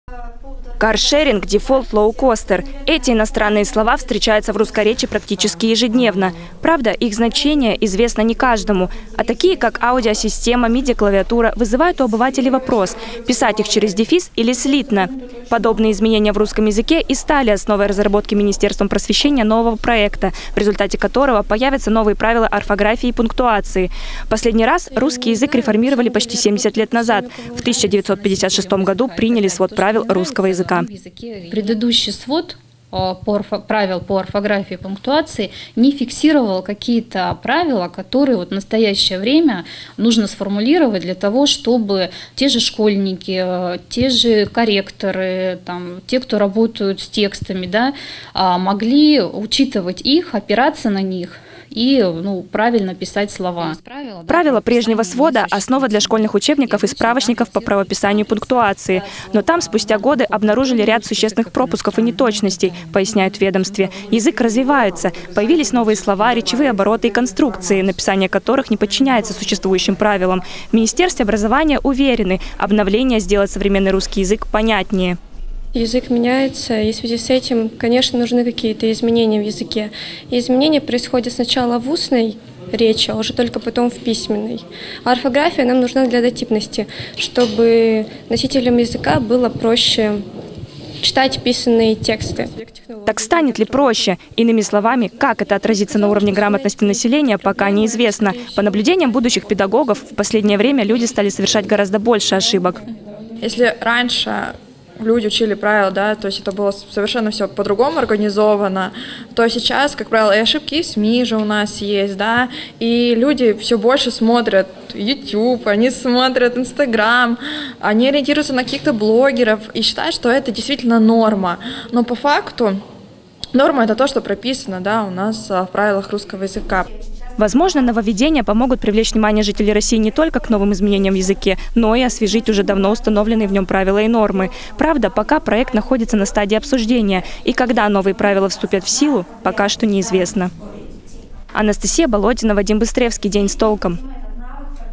Министерство просвещения России решило обновить правила русского языка
Министерство просвещения России решило обновить правила русского языка – такая новость появилась на днях в СМИ. Сообщается, что нововведения могут вступить в силу в ближайшее время. Что изменится  для рядовых граждан и почему русские люди часто говорят и пишут безграмотно?